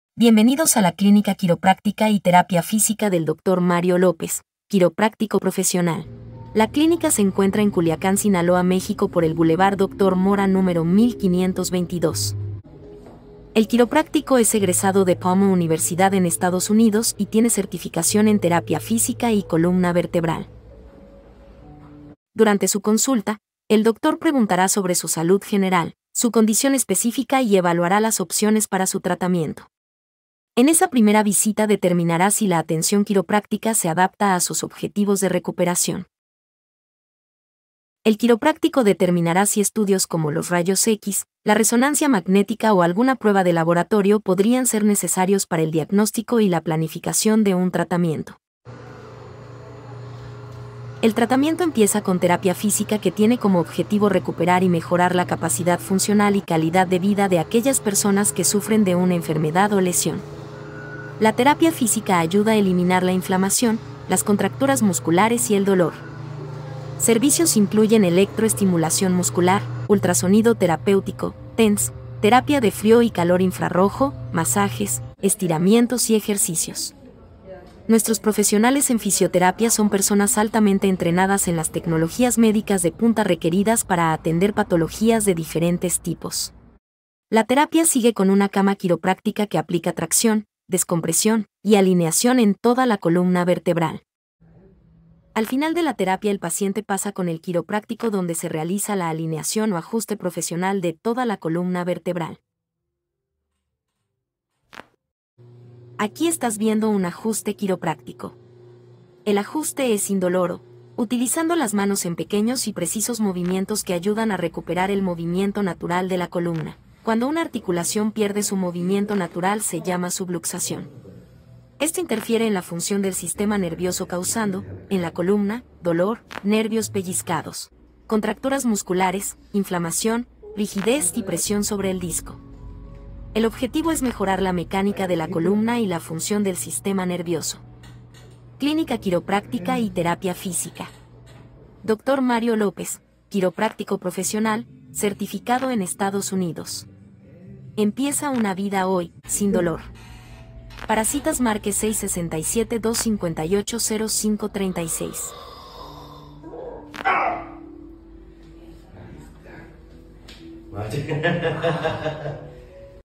Una Consulta y Terapia Completa en Clínica Quiropráctica y Terapia Física
En este video, acompáñanos mientras mostramos cada paso del proceso de consulta, desde tu llegada hasta el final de la terapia. 👀 Playlist de Ajustes

Comenzaremos con una evaluación inicial, donde nuestro equipo de profesionales revisará tu historial médico y realizará un examen físico detallado para comprender tus necesidades y preocupaciones específicas. Luego, te explicaremos el plan de tratamiento personalizado que hemos diseñado para ti, que puede incluir ajustes quiroprácticos, terapia física y ejercicios específicos.

Durante la sesión de terapia, podrás ver cómo aplicamos técnicas avanzadas para aliviar el dolor y mejorar la movilidad. Te mostraremos cómo cada enfoque se adapta a tus necesidades individuales, garantizando una experiencia positiva y transformadora.

Nuestro objetivo es proporcionarte las herramientas necesarias para alcanzar un bienestar óptimo y ayudarte a vivir una vida activa y saludable.